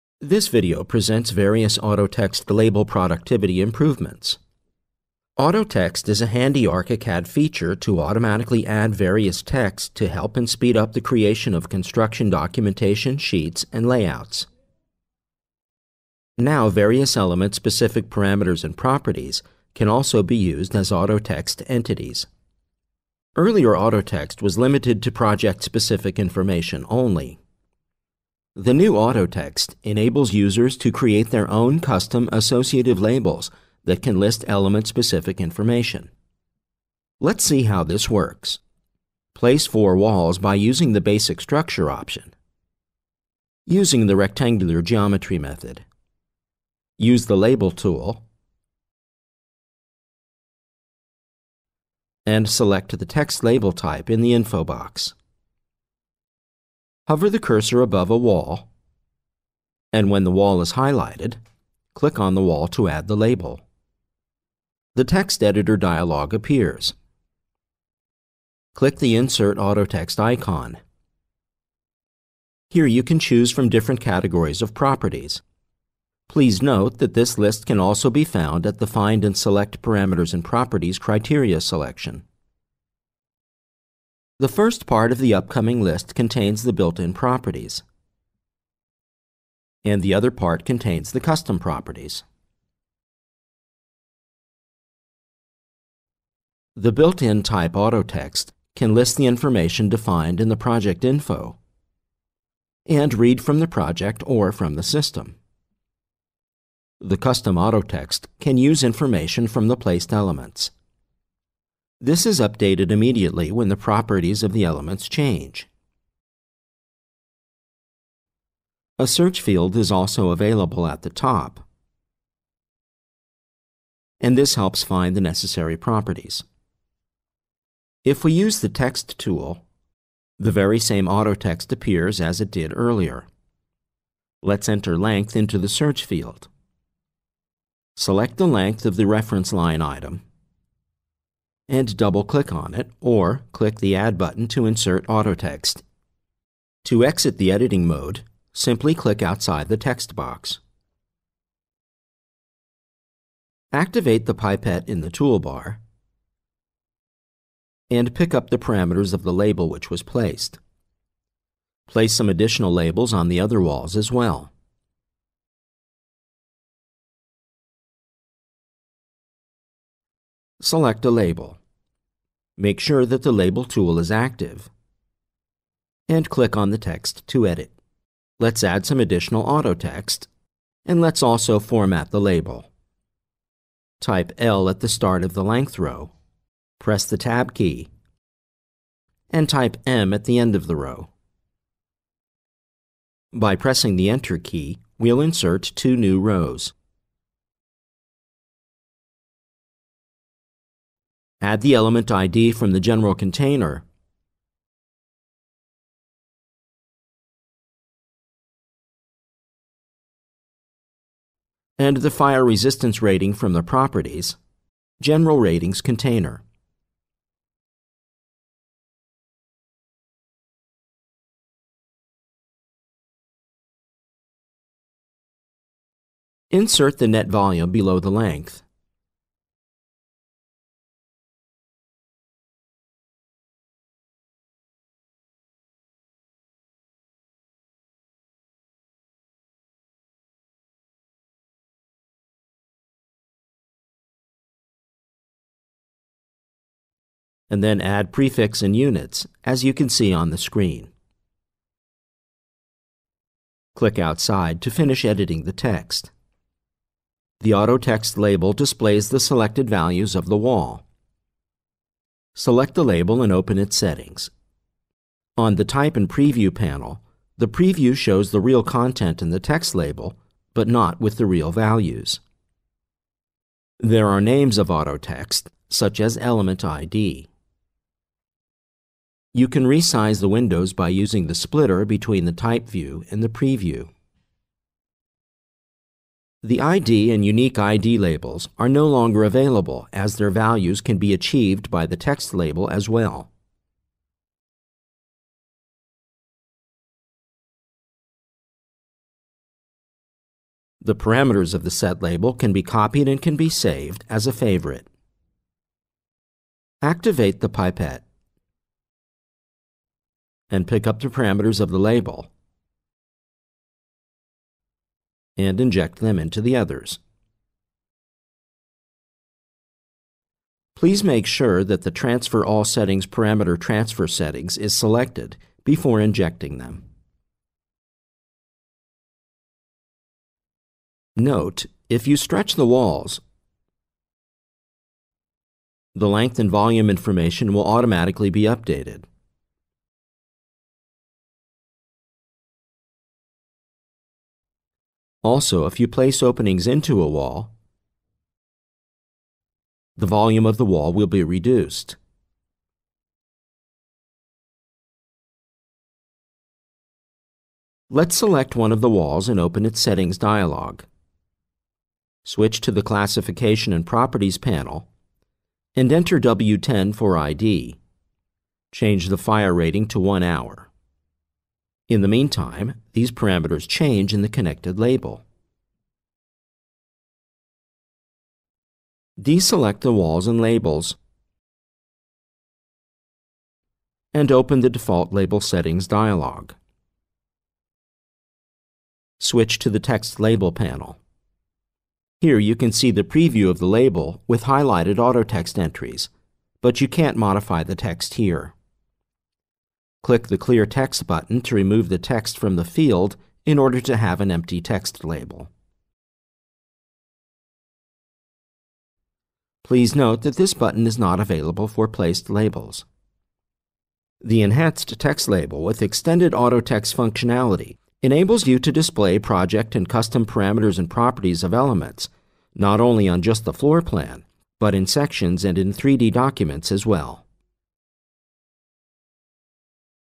ARCHICAD 21 - Autotext Label Enhancements
This video presents various Autotext Label productivity improvements. Autotext is a handy ARCHICAD feature to automatically add various texts to help and speed up the creation of construction documentation sheets and layouts. The Enhanced Text Label with extended Autotext functionality, enables you to display project and custom Parameters and Properties of elements not only on just the floor plan, but in sections and in 3D documents as well.